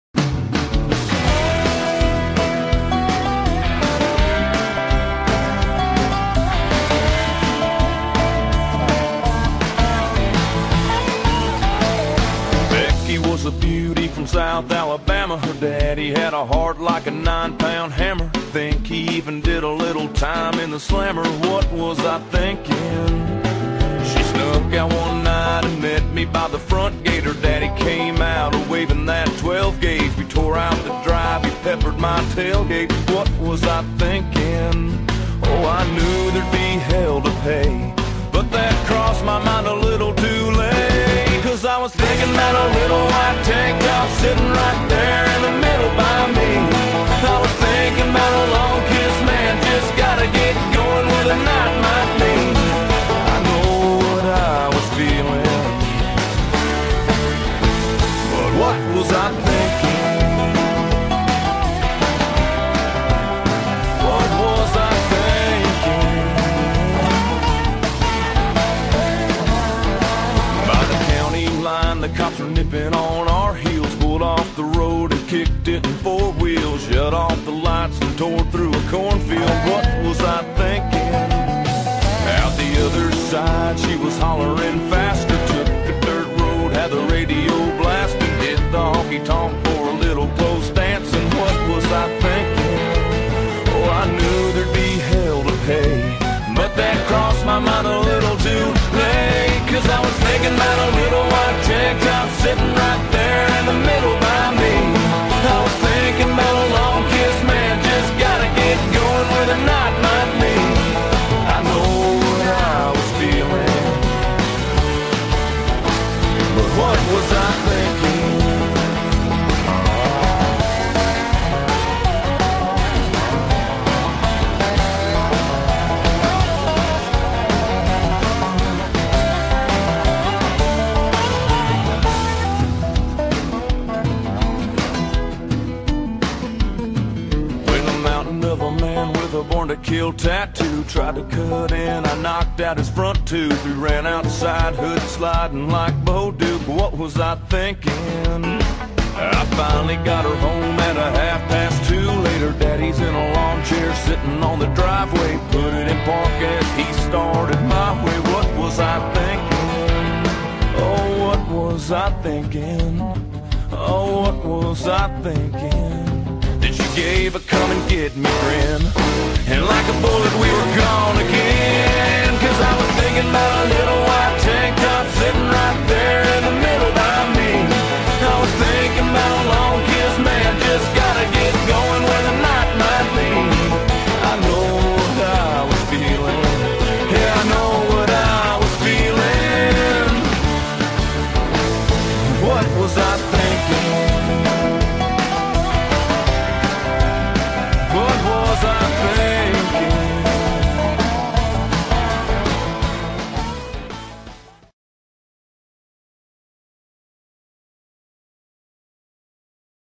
Thinkin' Country - Line Dance
48C, 4W, Beginner Level Line Dance
Choreographed by Simon Ward, Aust. - Jan 2016